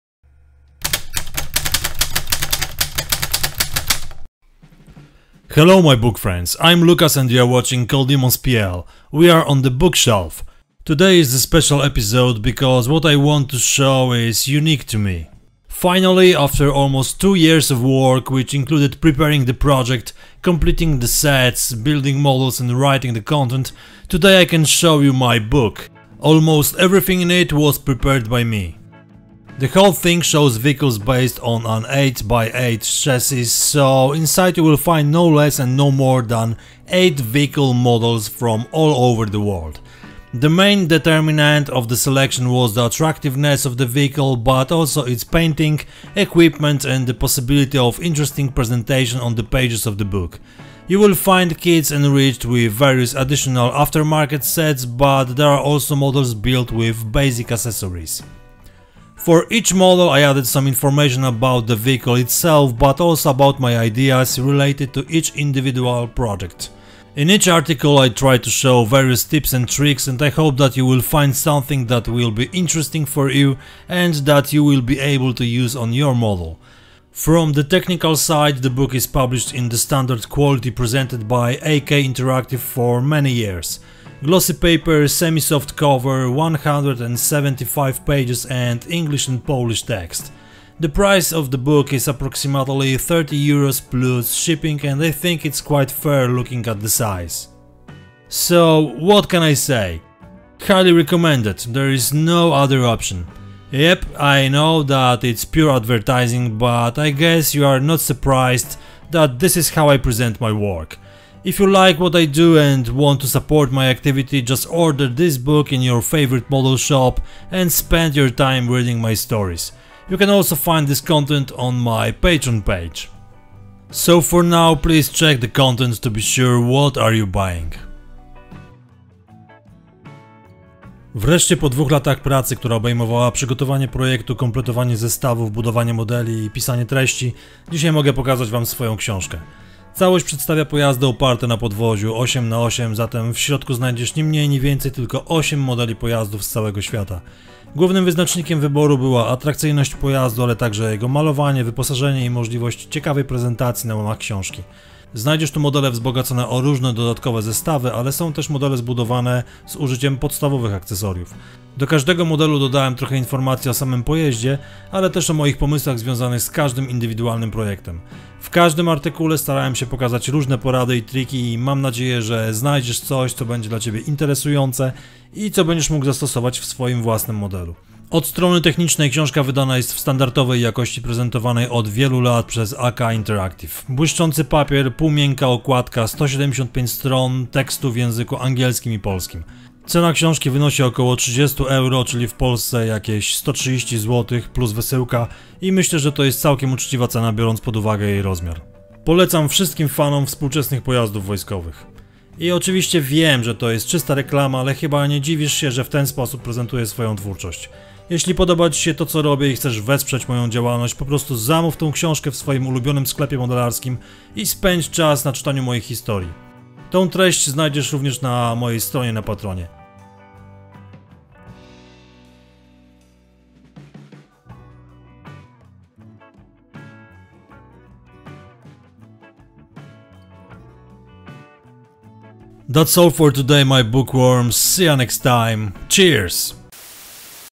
BOOKSHELF - Modeling Modern Armored Fighting 8x8 Vehicles, AK Interactive, AK 130017
Today in Bookshelf series you can check MY BOOK - Modeling Modern Armored Fighting 8x8 Vehicles from AK Interactive (AK130017)
This book is entirely dedicated to modern 8x8 vehicles. Here you will find impressive machines used by armies around the world. Together with the STRYKER M1132, B1 CENTAURO and KTO ROSOMAK you will also be able to see a BOXER AMBULANCE, VBCI, VPK-7829 BUMERANG, ZTL-11 and an ASLAV-PC PHASE 3.
In each of them you will find different techniques that you can transfer to other models. Each article shows step-by-step the construction, painting and weathering of each model, as well as the preparation of the base. The book shows various camouflage patterns, as well as the finishing of the models in different levels of detail.